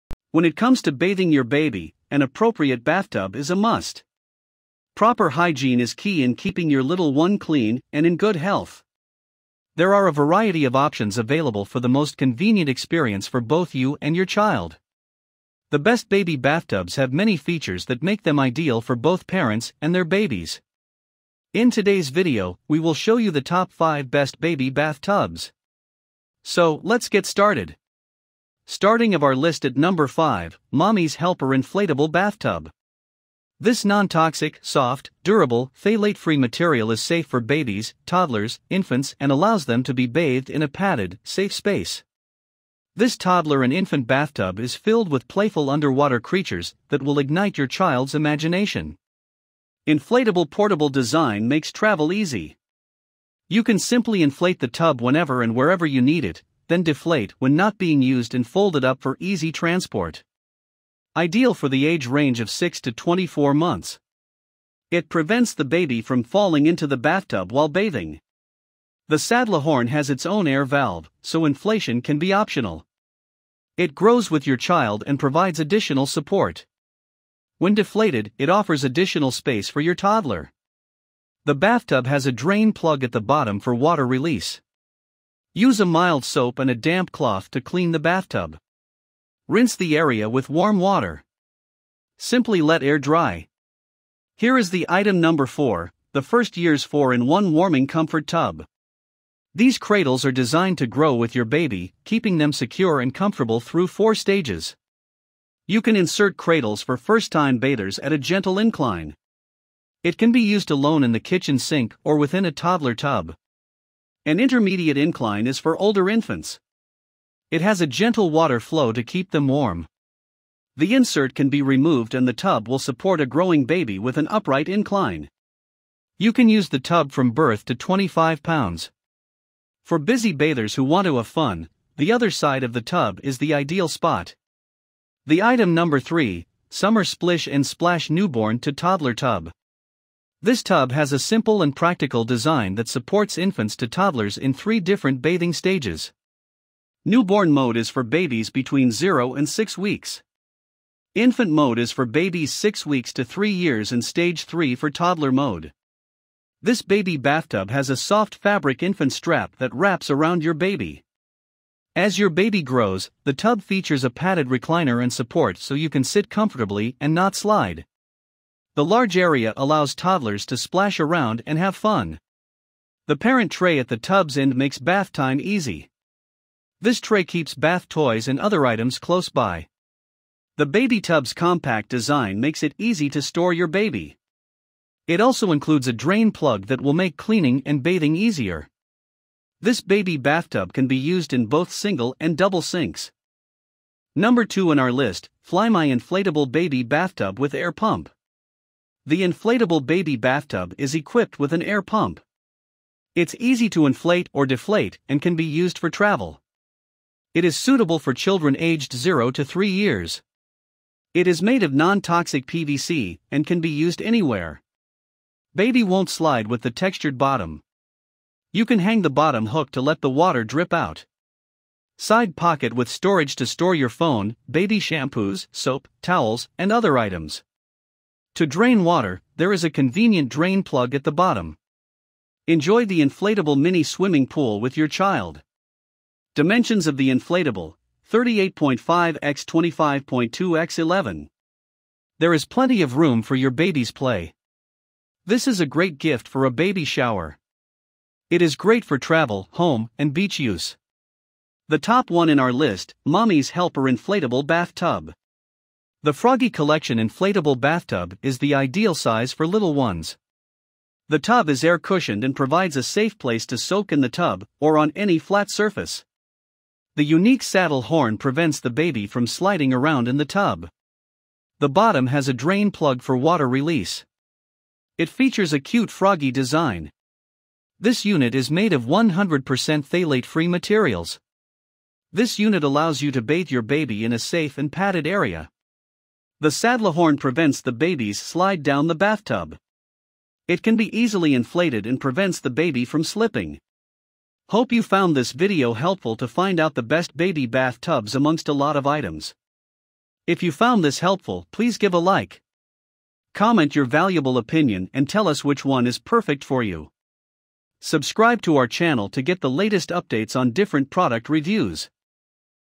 Best Baby Bath Tub 2021
5. Mommy's Helper Inflatable Bathtub

4. The First Years 4 in 1 Warming Comfort Tub

3. Summer Splish 'n Splash Newborn to Toddler Tub

2. FLYMEI Inflatable Baby Bathtub with Air Pump

1. Mommy's Helper Inflatable Bath Tub